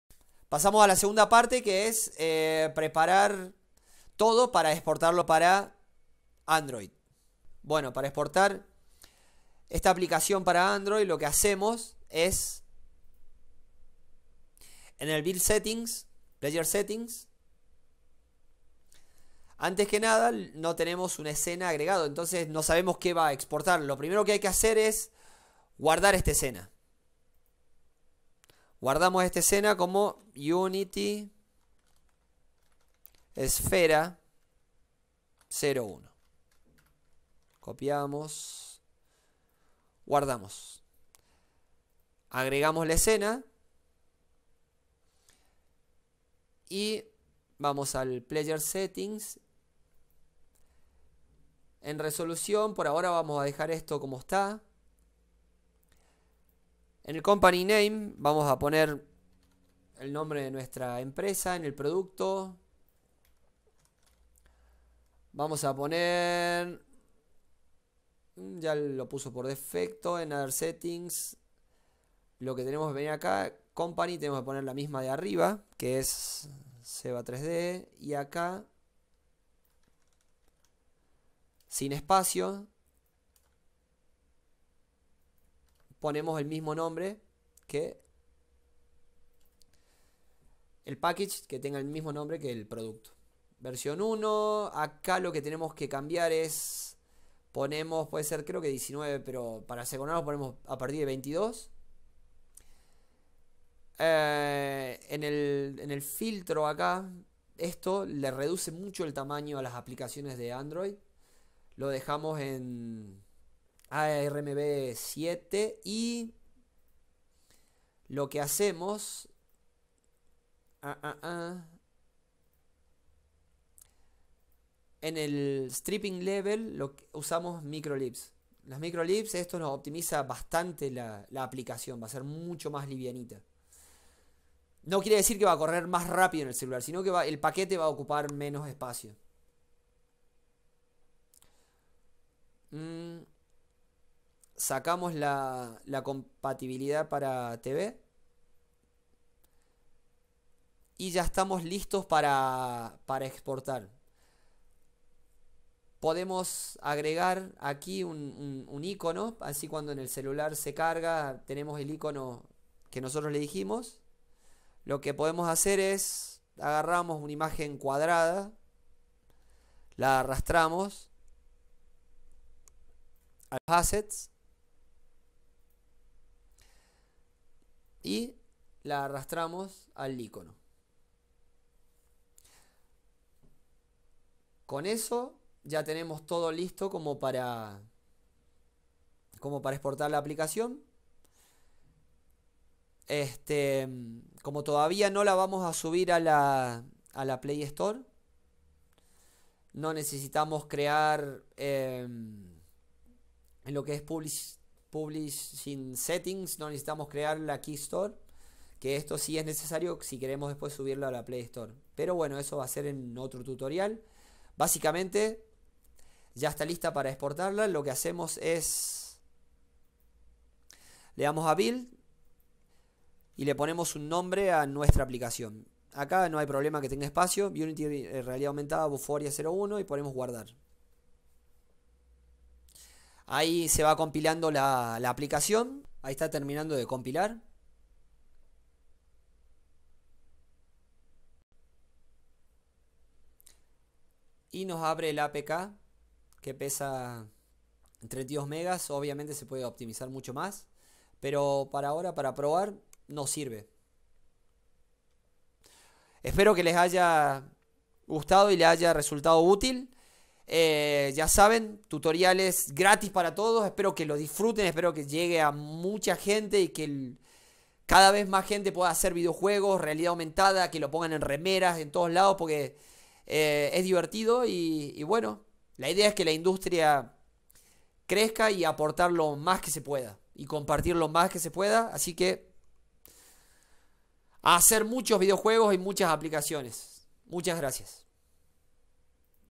Seba3d Realidad Aumentada Con Vuforia Tutorial Parte 02
Tutorial para crear el APK para usar en dispositivos con Android.